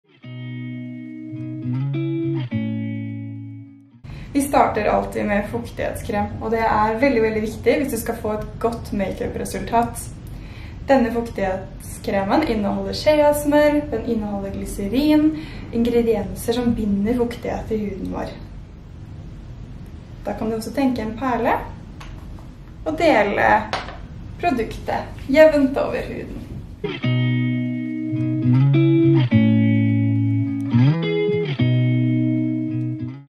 SKIN CARE - DAY CREAM! Dagkrem gir fuktighet og næring til huden.
Undertekst: Norsk
SKIN CARE by Makeup Mekka
HYDRA SKIN - DAY CREAM

En lett fuktighetskrem som trekker raskt inn i huden, dagkremen skaper et perfekt grunnlag for sminke.
Dagkremen gjør huden myk og holder på fuktigheten hele dagen. Inneholder ingredienser som gir fuktighet og bidrar til en fresh hud. Som for eksempel glycerin, sheasmør og squalane som bidrar til næring og fukt.
Bruke dagkrem som nattkrem? Hydra Skin Day Cream er en dagkrem som også kan brukes som nattkrem.